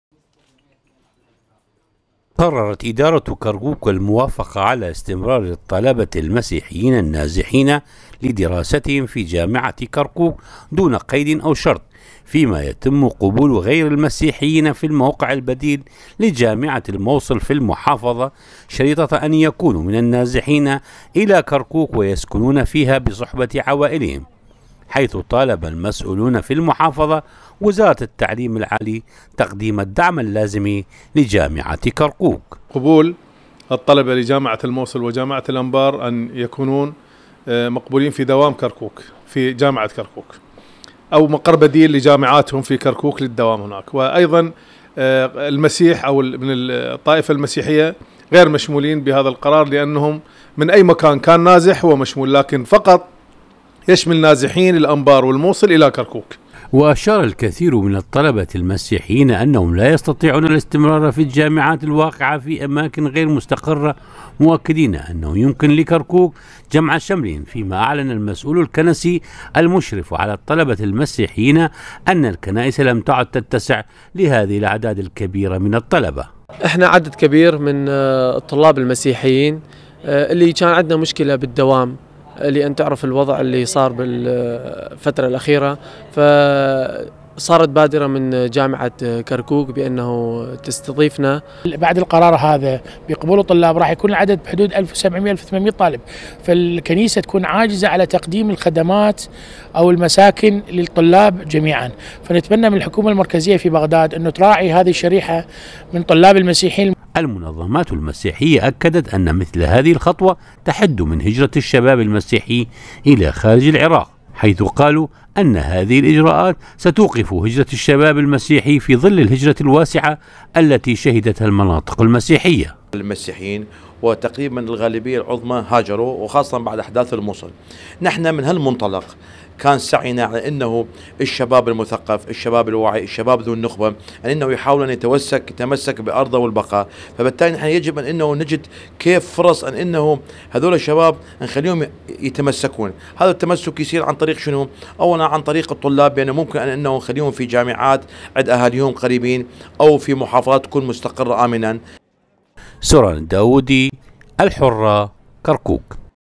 قبول الطلاب المسيحيون المهجرون في جامعة كركوك
قررت ادارة كركوك الموافقة على استمرار الطلبة المسيحيين النازحين  في جامعة كركوك دون قيد او شرط فيما يتم قبول غير المسيحيين في الموقع البديل لجامعة الموصل في كركوك  شرط ان يكونو من النازحين الى كركوك ويسكنون فيها  وقد طالب الطلبة بتامين اقسام داخلية لهم واعلن السئول الكنسي  المشرف على الطلبة المسيحيين ان الكنائس لا تتسع لهذه الاعداد الكبيرة من الطلبة  . فيما المسيحيون  قالو ان هذا الاجراء سيوقف هجرة  الشباب المسيحيي